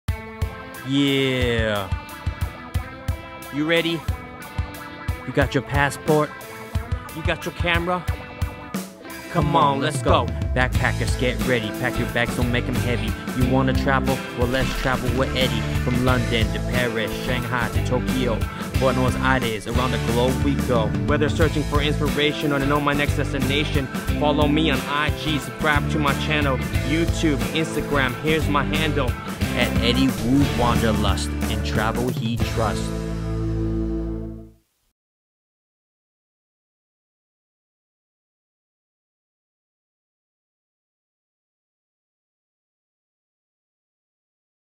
Eddie Woo Wanderlust Channel Intro
Welcome to Eddie Woo Wanderlust, now playing on YouTube! On this channel you will find videos on all the things I'm passionate about. Traveling, filmmaking, technology, culture, and food. You will also see my free spirit. These are all the things I love and make me who I am.
And by the way, that is me rapping!